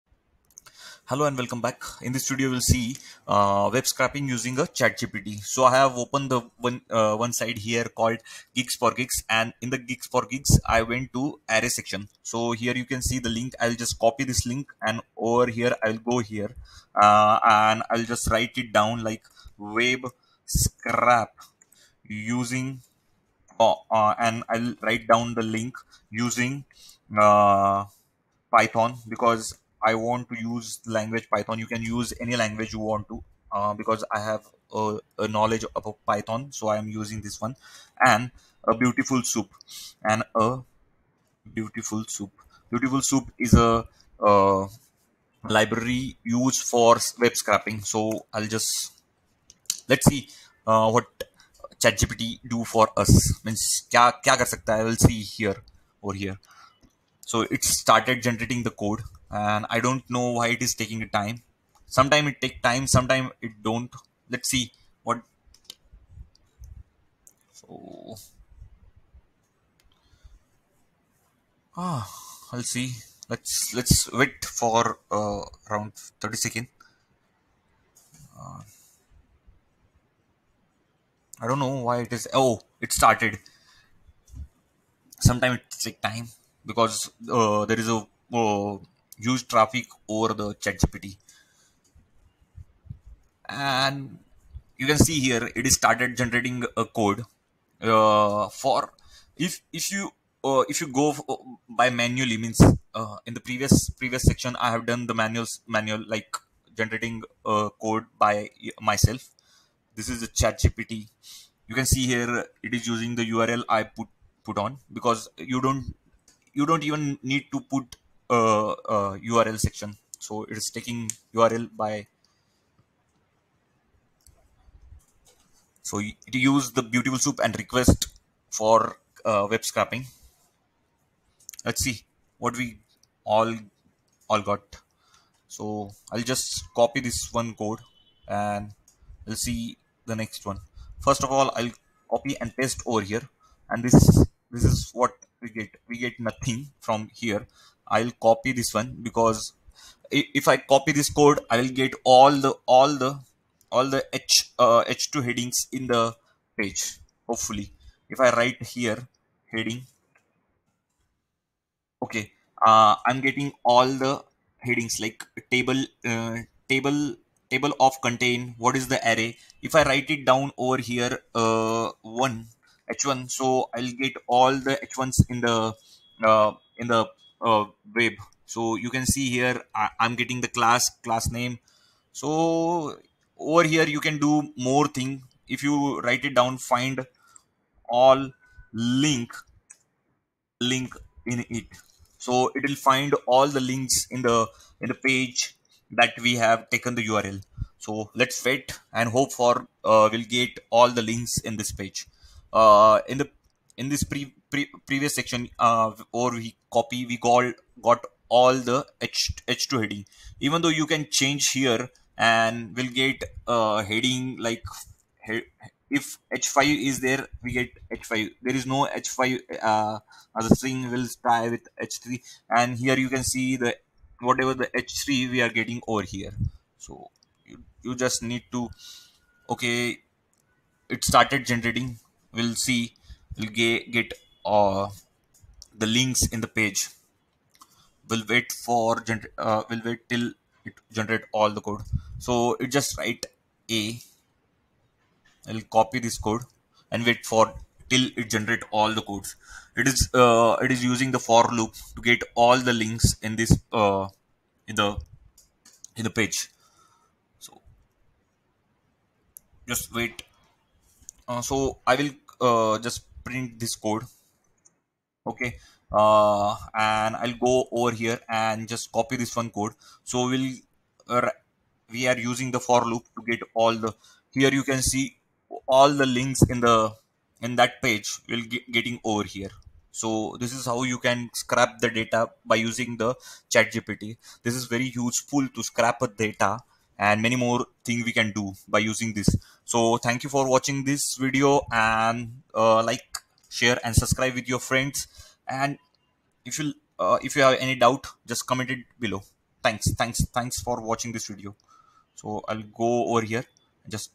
Web scraping art using Chatgpt
Used python, BeautifulSoup, request for web scraping